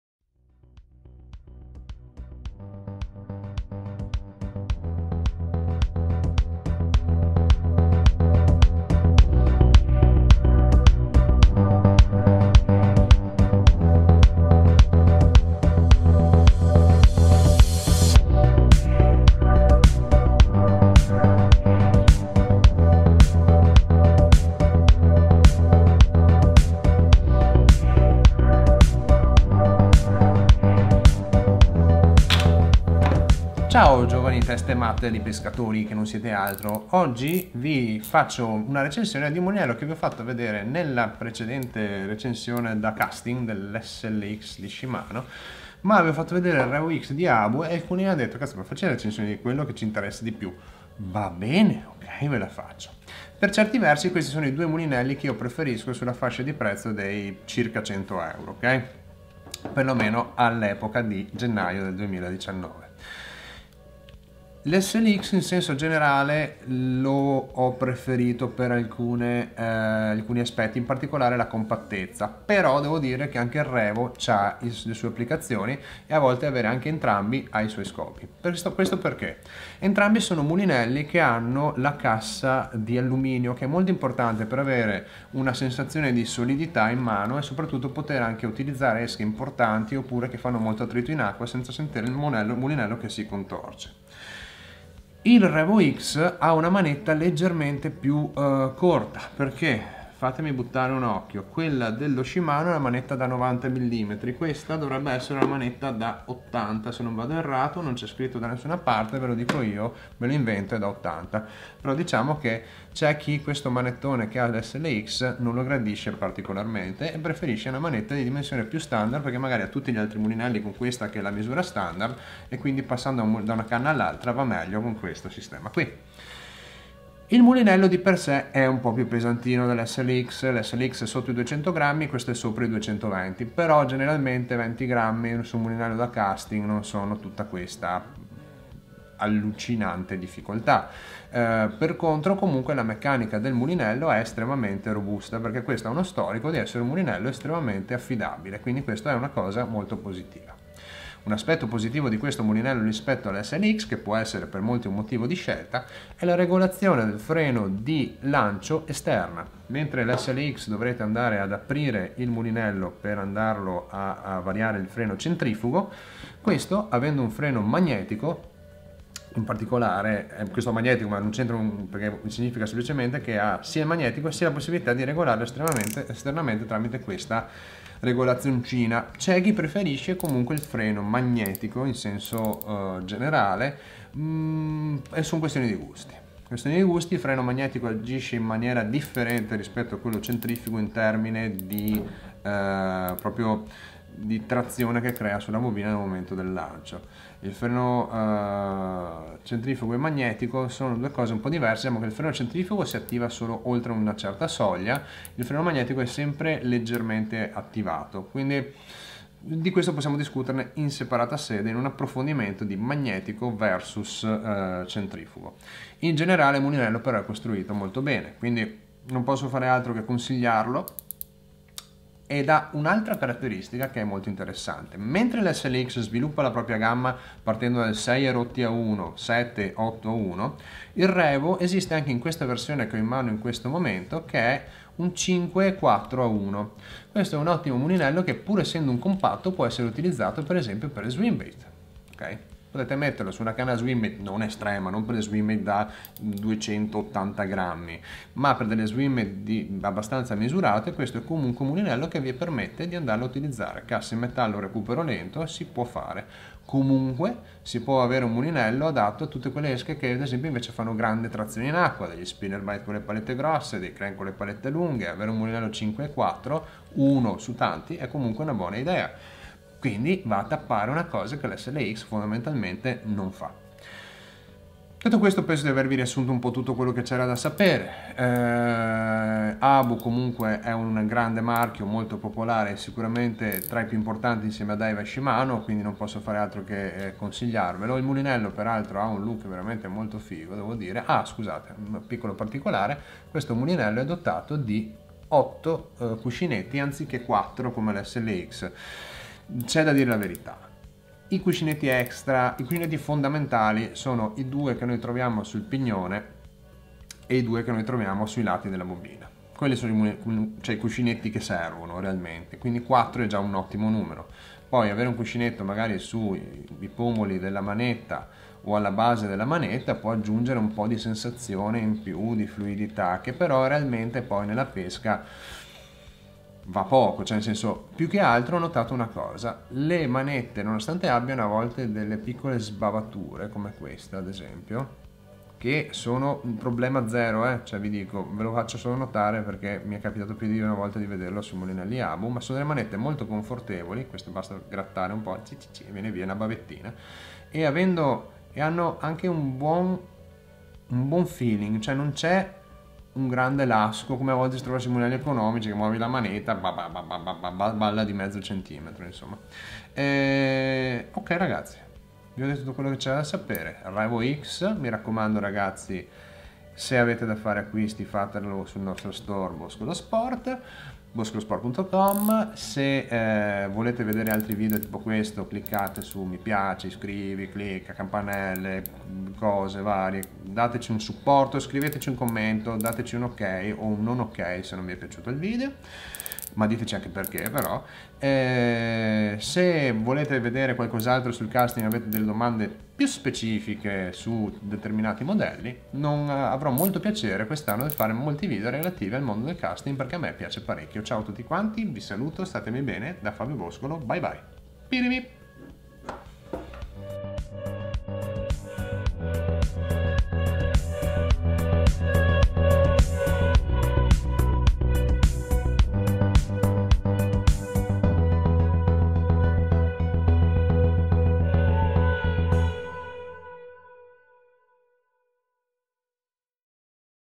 ABU REVO-X : RECENSIONE MULINELLO CASTING
Abu REVO-X : è solido, nero, e ha caratteristiche interessanti. Il mulinello da casting di Abu ha le carte in regola per battere lo Shimano SLX sulle stesse fasce di prezzo?

Shimano SLX: Miglior mulinello casting sotto i 100€?

Questo mulinello che si pone come erede del Casitas che era già un ottimo prodotto. Ma questo SLX aggiunge un frame in alluminio (pur levando il freno ad accesso esterno) ed una manetta pari a quella del Curado K.

Decisamente un ottimo prodotto!